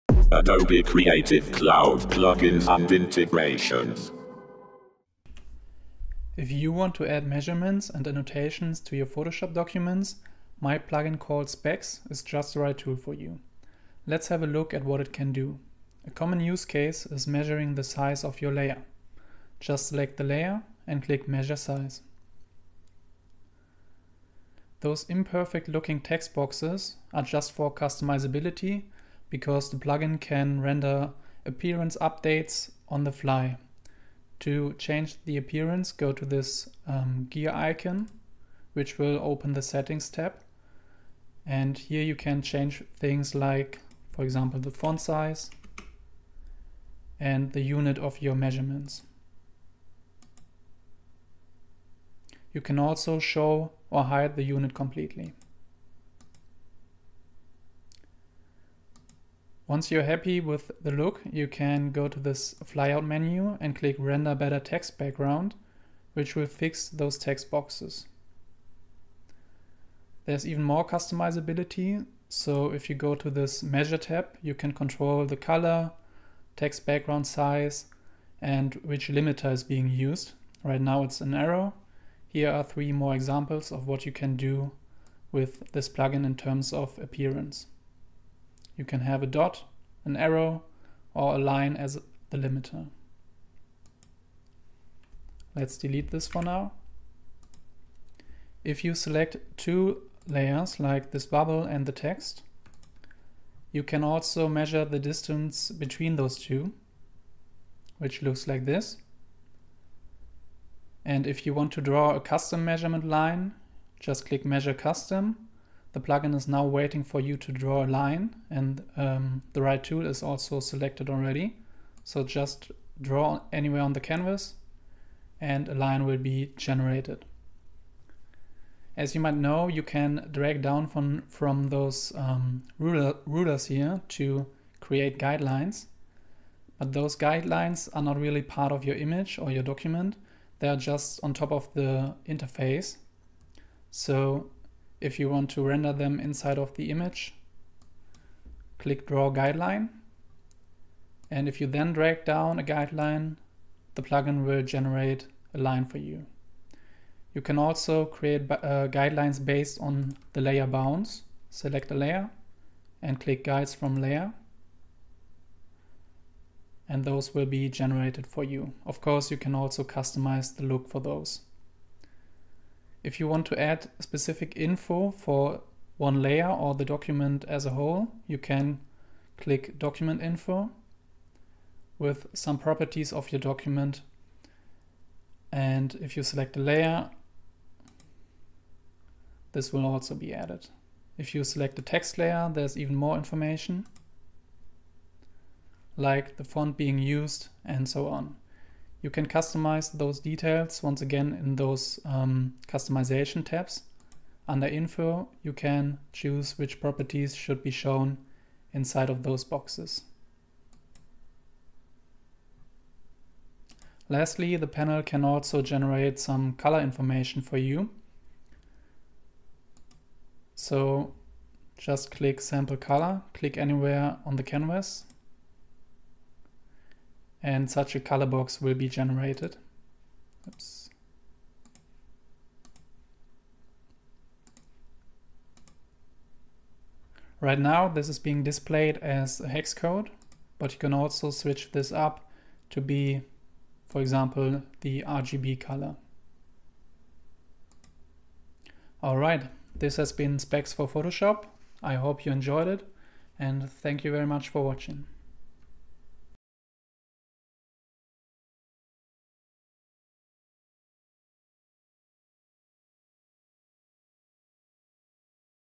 Specs by Simon Henke | Plugin Demo | Adobe Creative Cloud
Specs makes it easy to add customisable measurements, annotations and more to Photoshop documents, which is often useful for print and UI designers.